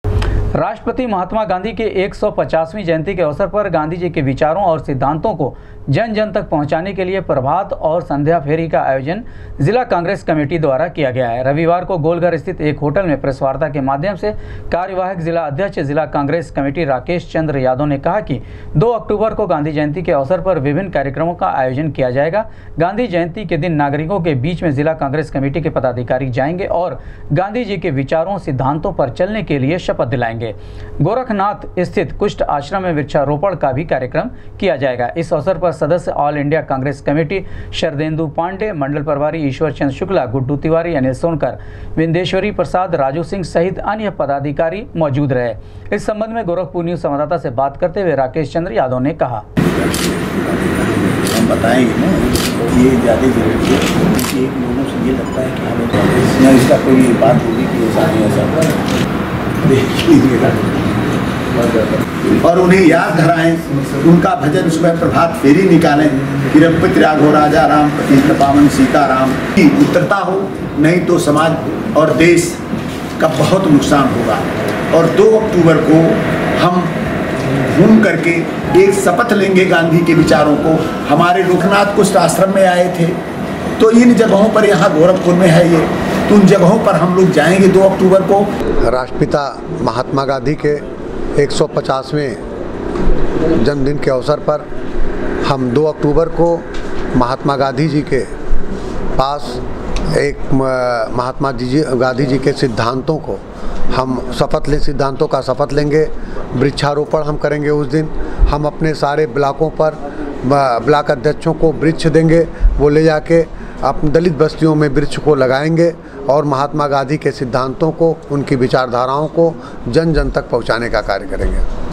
कांग्रेस के कार्यवाहक जिलाध्यक्ष ने कहा, गांधी जयंती पर गांधीजी के सिद्धांतों और विचारों पर चलने
कांग्रेस के कार्यवाहक जिलाध्यक्ष ने कहा, गांधी जयंती पर गांधीजी के सिद्धांतों और विचारों पर चलने की दिलाएंगे शपथ, वृक्षारोपण कर समाज को देंगे संदेश